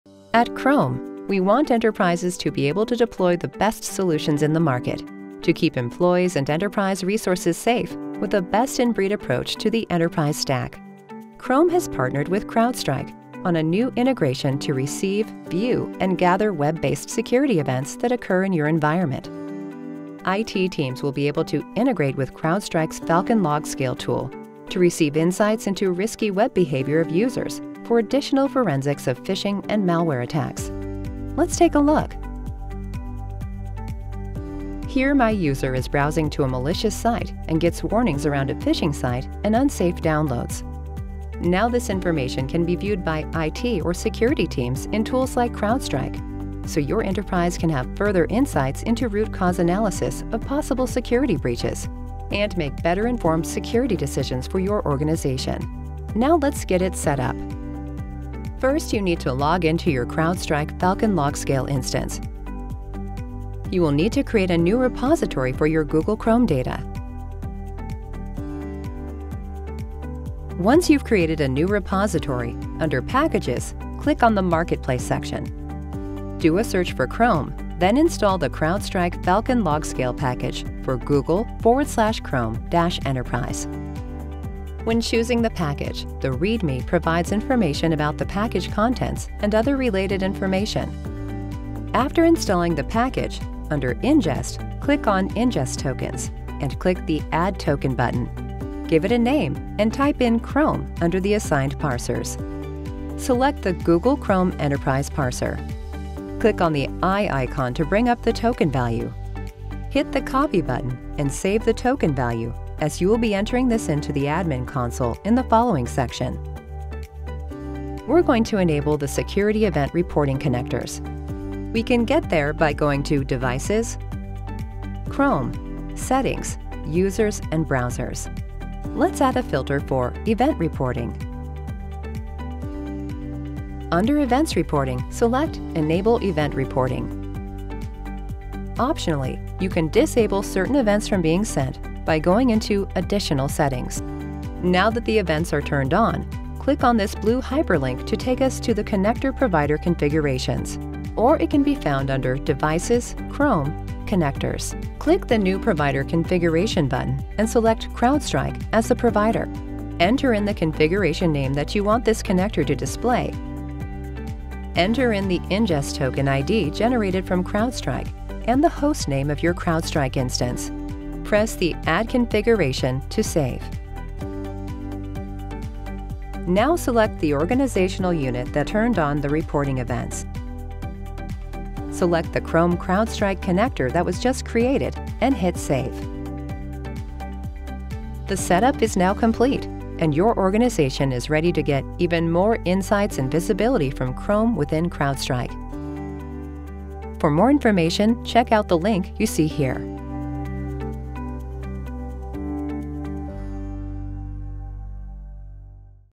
Chrome’s Security Insights Extended to CrowdStrike
With security being a top priority, Chrome has partnered with CrowdStrike on a new integration to collect, analyze, and extract insights from security events through the Chrome Enterprise Connector Framework. With the extended security insights into potential web-based threats, IT and security teams will have the data they need to make better-informed security decisions.

You can set up this integration within minutes in Chrome Browser Cloud Management at no additional cost if you're currently a CrowdStrike user. Get started at g.co/chromecloudmanagement